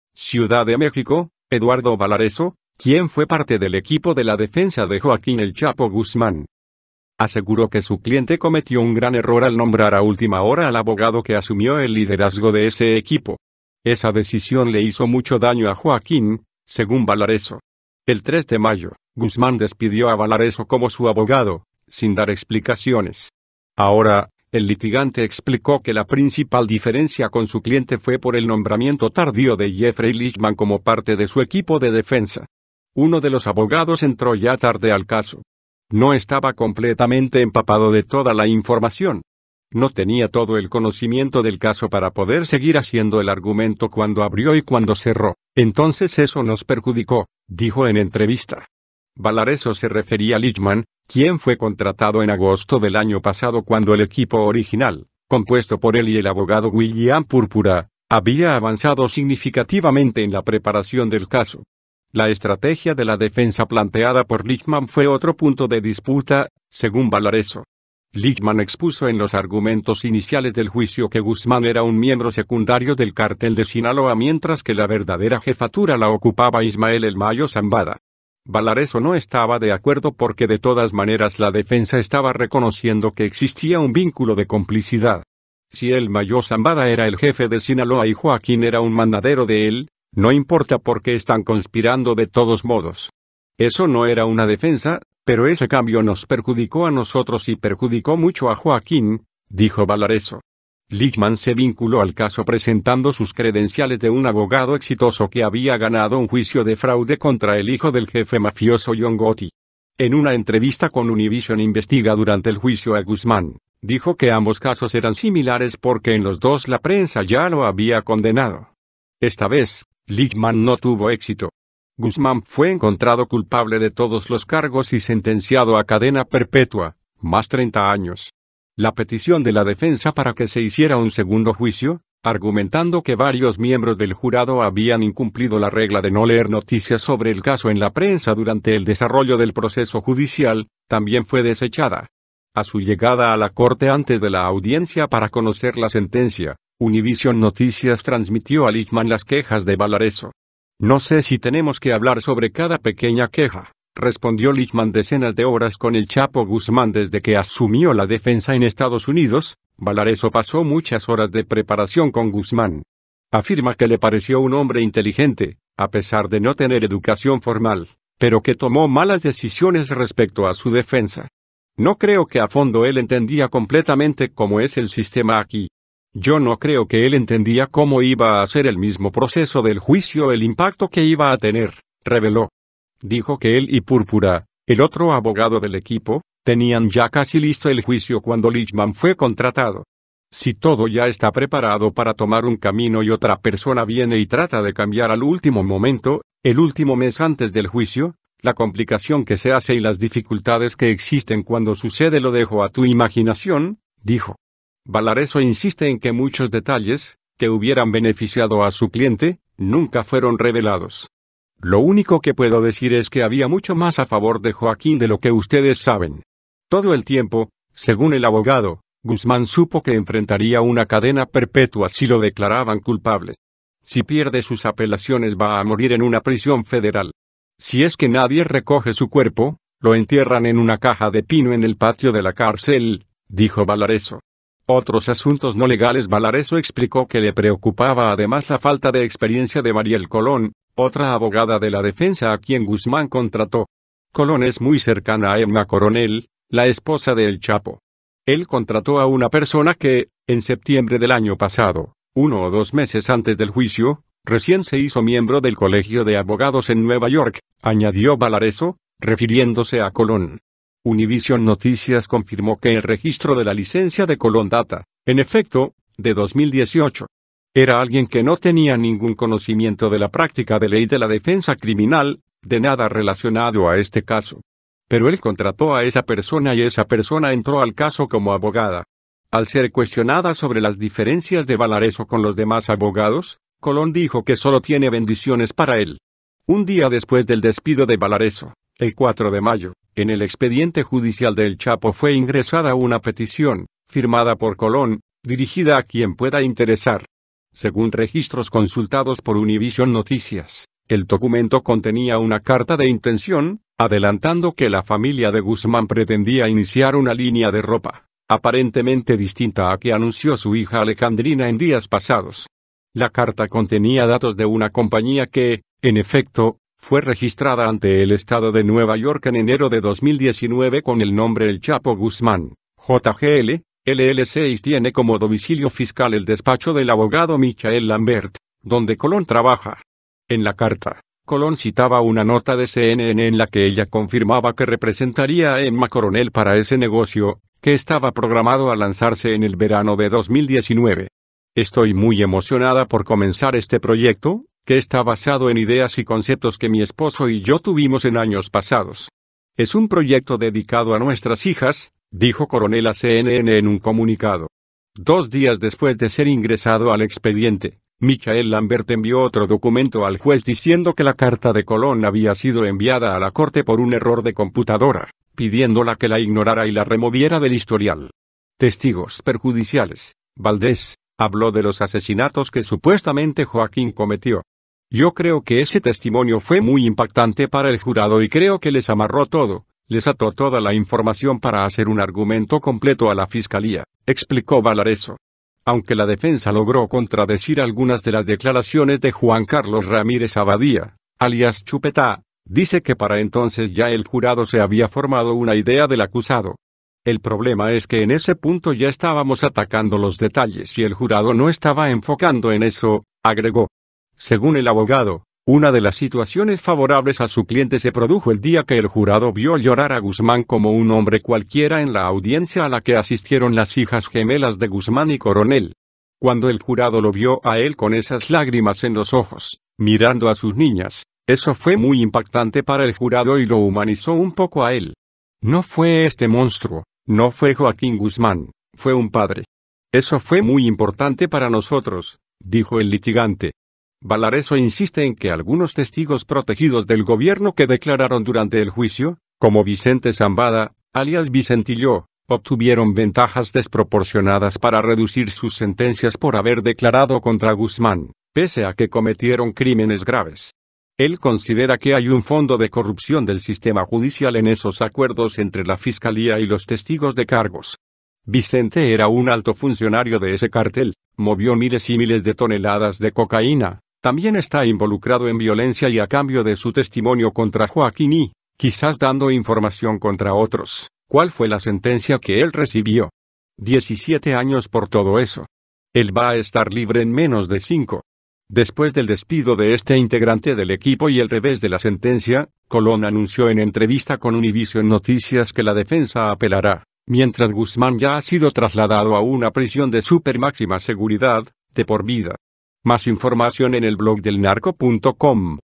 Revelan el gran error de “El Chapo” el exlíder del Cartel de Sinaloa
Ciudad de México – Eduardo Balarezo, quien fue parte del equipo de la defensa de Joaquín “El Chapo” Guzmán.

Aseguró que su cliente cometió un gran error al nombrar a última hora al abogado que asumió el liderazgo de ese equipo.  Esa decisión le “hizo mucho daño a Joaquín”, según Balarezo.  El 3 de mayo, Guzmán despidió a Balarezo como su abogado, sin dar explicaciones. Ahora, el litigante explicó que la principal diferencia con su cliente fue por el nombramiento tardío de Jeffrey Lichtman como parte de su equipo de defensa.